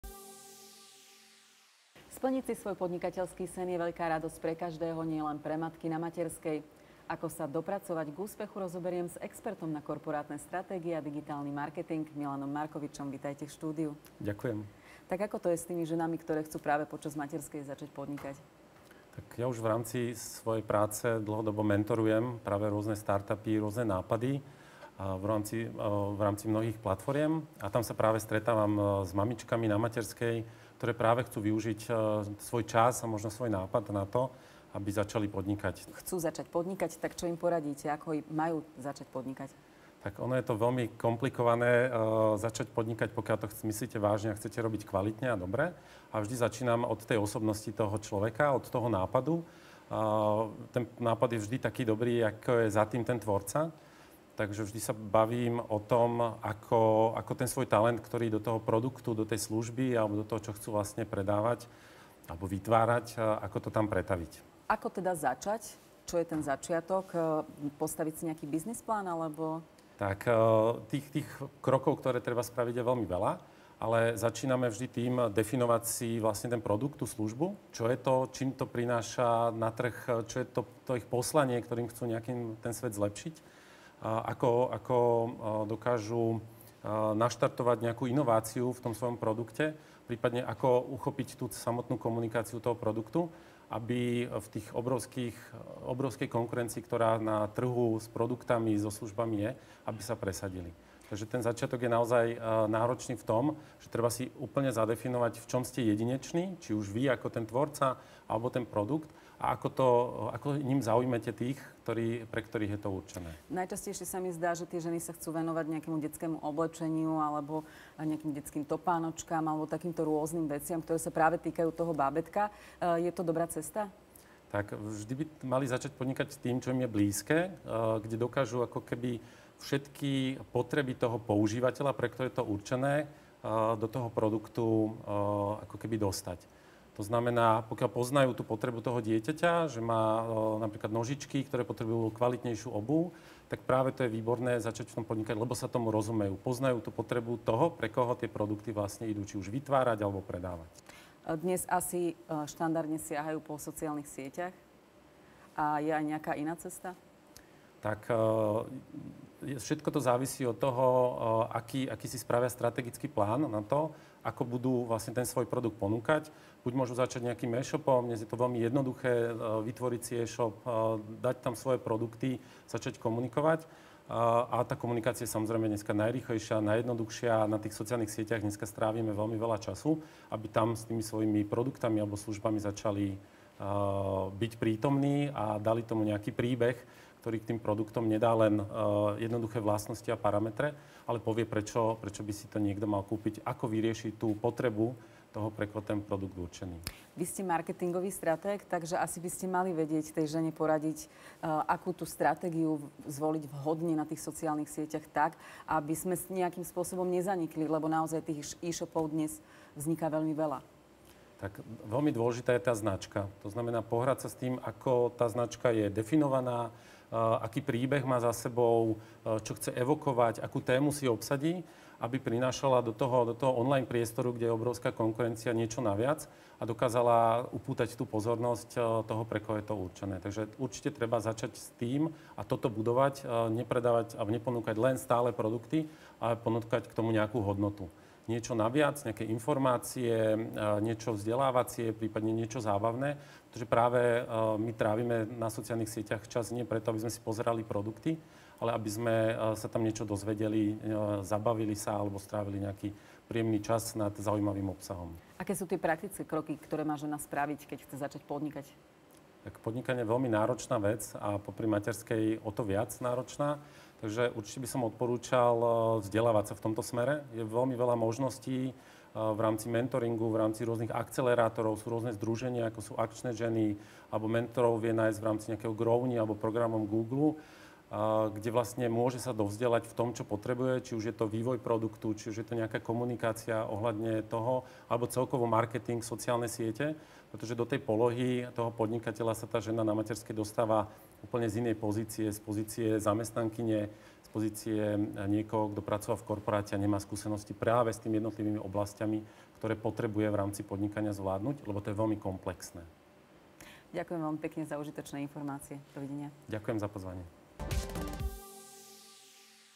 Milan Markovič: Ako úspešne podnikať na materskej
Splniť si svoj podnikateľský sen je radosť pre každého, nielen pre matky na materskej. Ako sa dopracovať k úspechu, sme rozobrali s expertom na korporátne stratégie a digitálny marketing, Milanom Markovičom z Effectix.